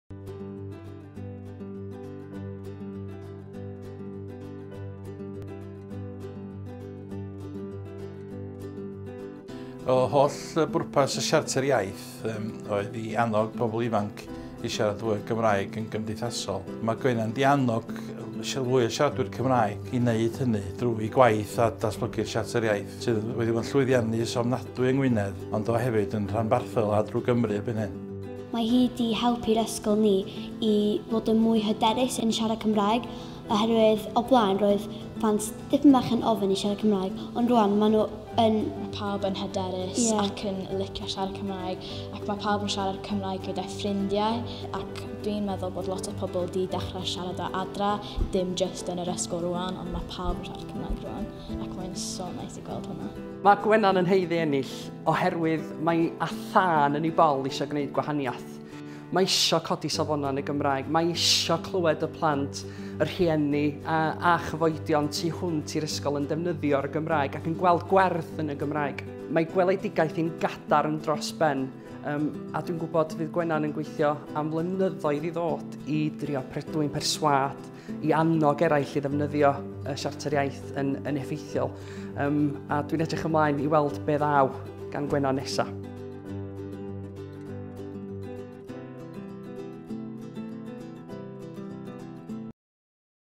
Gwenan Ellis Jones - Defnyddio'r Gymraeg .../Inspirational use of Welsh
Enillydd y Wobr Defnyddio'r Gymraeg mewn ffordd sy'n ysbrydoli 2018
Winner of the Inspirational use of the Welsh Language 2018